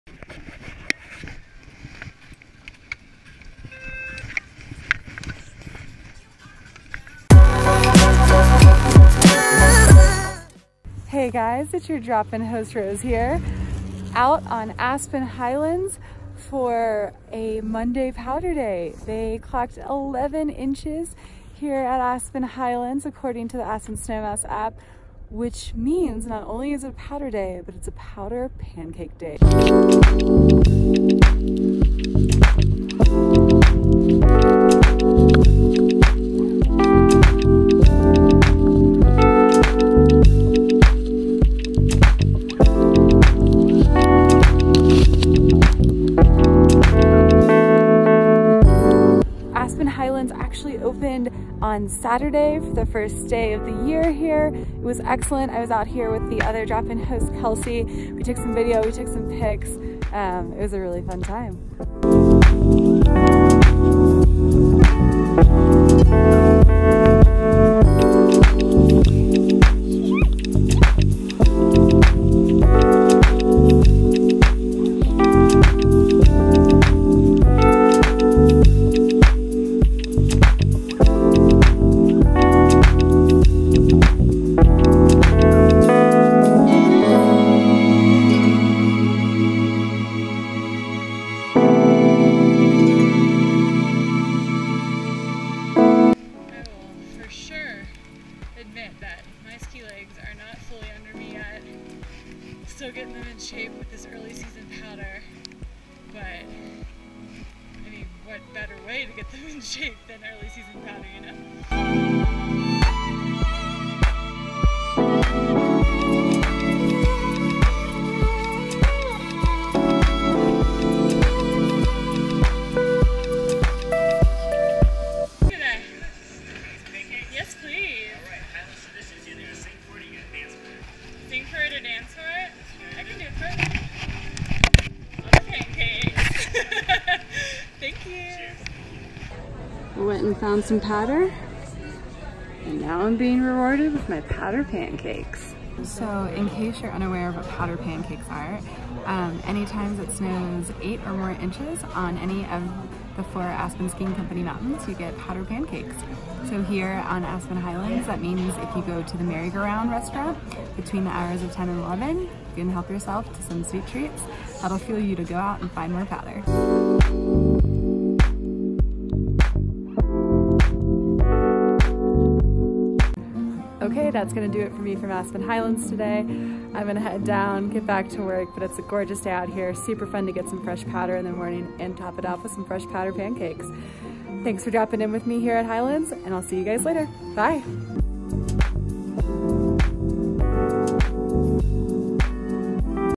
The Drop-In: Powder & Pancakes at Aspen Highlands
In the past 24 hours, Aspen Skiing Co. measured 11 inches of fresh snowfall at Aspen Highlands, making Monday, Dec. 9, a powder pancake day!